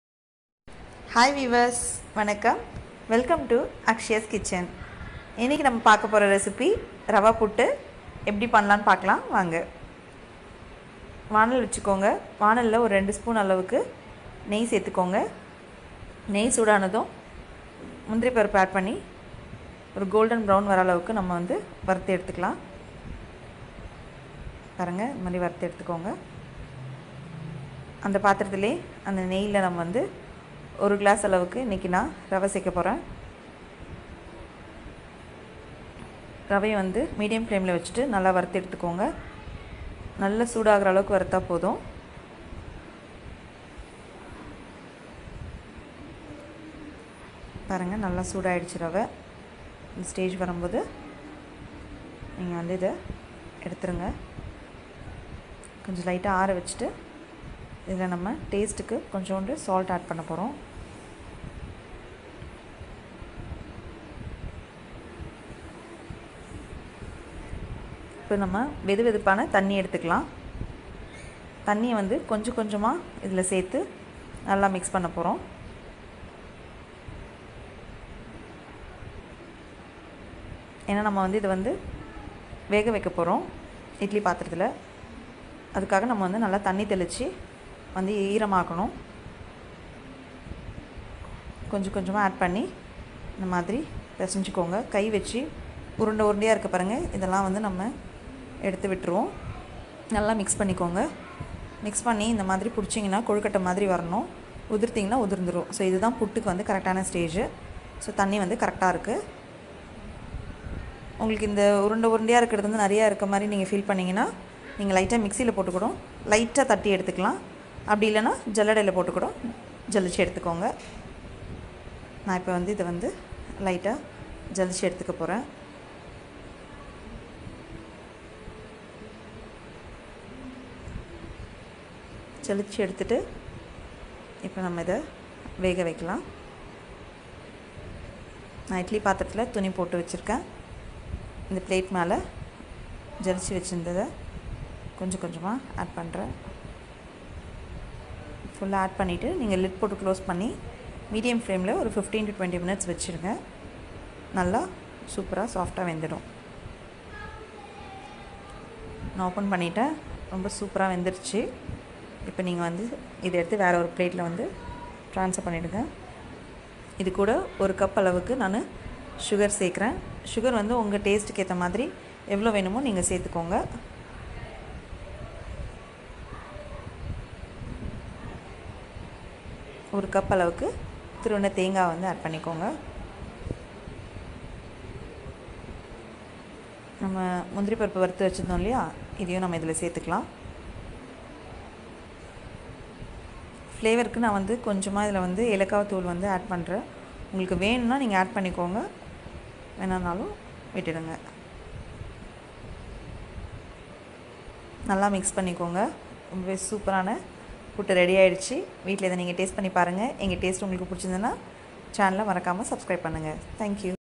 சுவையாக ரவை புட்டு | Rava Puttu | Sweet & Soft sooji puttu/ Quick and easy Rava Puttu Snack Recipe/
ரவை இருக்கா டேஸ்டான ஒரு பிரேக்ஃபாஸ்ட் ரெடி /easy tiffin recipe
Rava puttu, a delicious breakfast or evening time snack. Easy to prepare in minimum time with few ingredients. Kids will love it.

Be Home, Be Safe .. Follow Government regulations to prevent spread of COVID-19